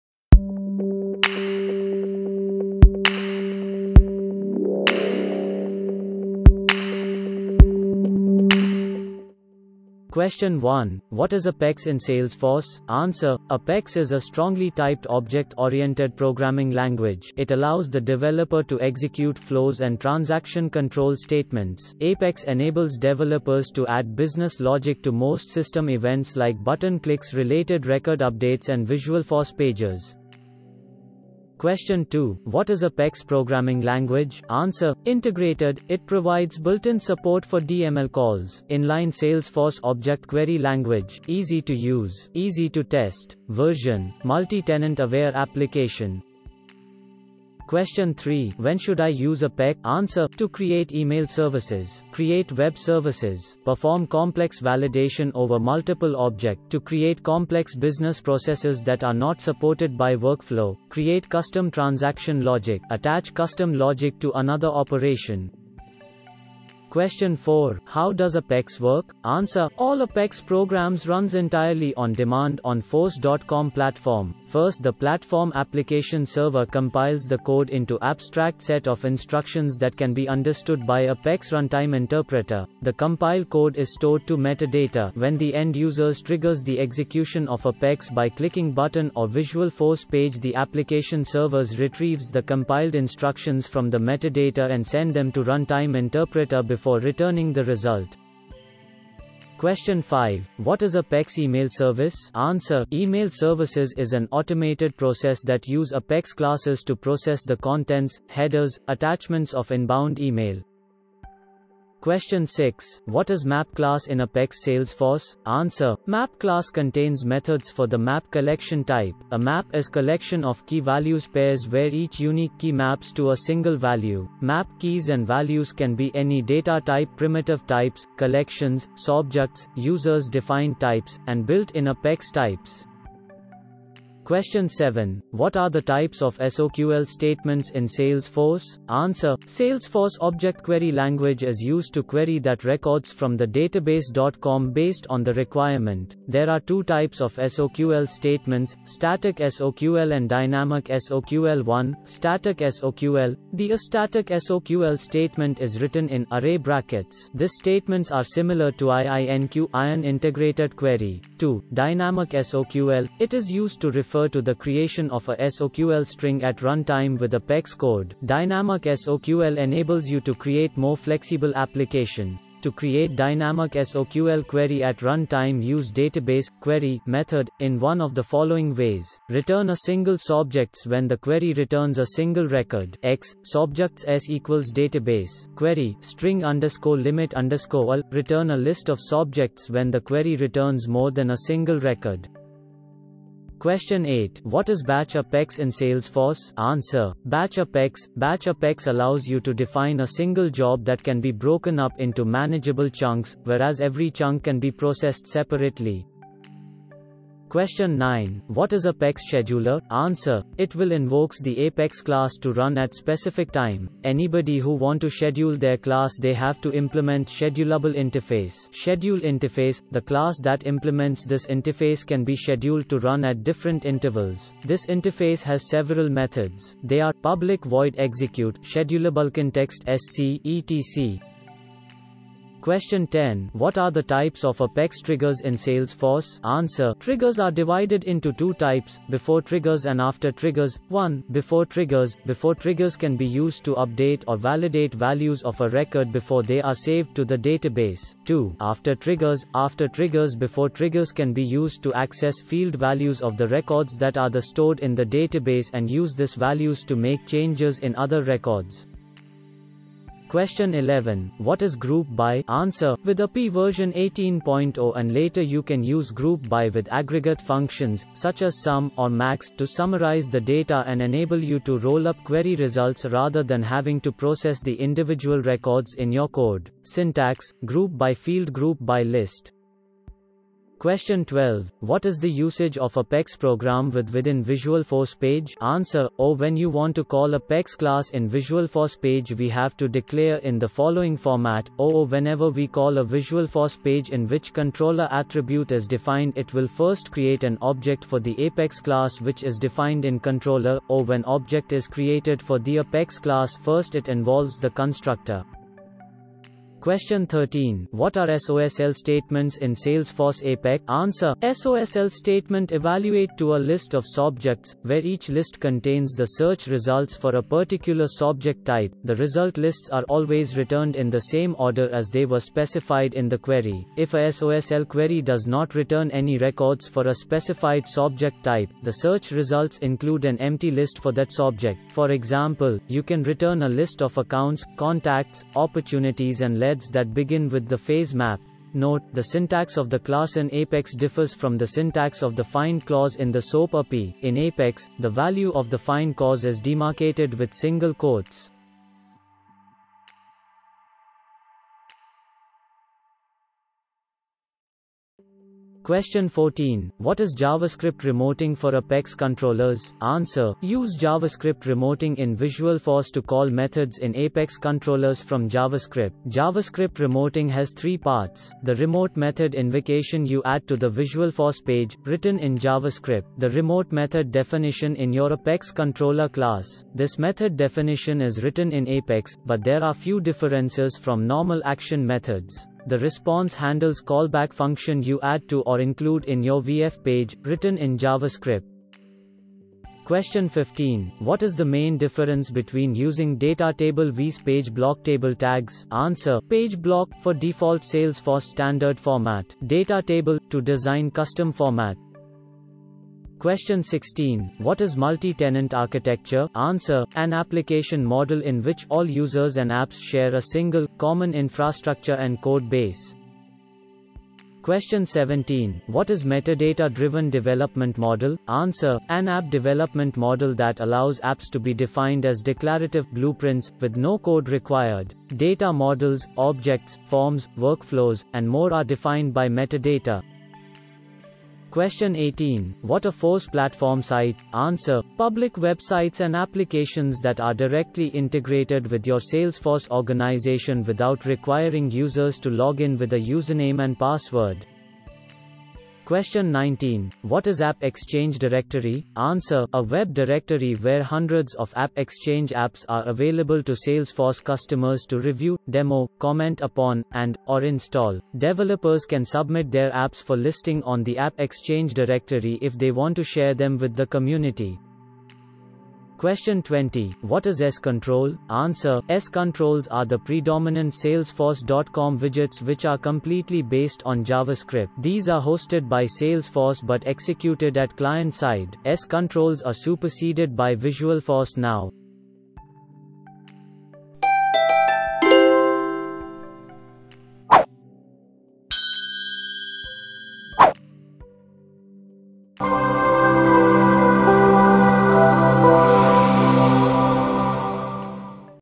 TOP 20 Salesforce Interview Questions and Answers 2019
20 Salesforce Interview Questions and Answers, Question1: What is Apex in Salesforce? Question2: What is Apex Programming Language? Question3: When Should I Use Apex? Question4: How Does Apex Work? Question5: What is Apex Email Service?

Join the LIVE session on Salesforce Interview Questions and Answers in your Technical round of Job Interviews.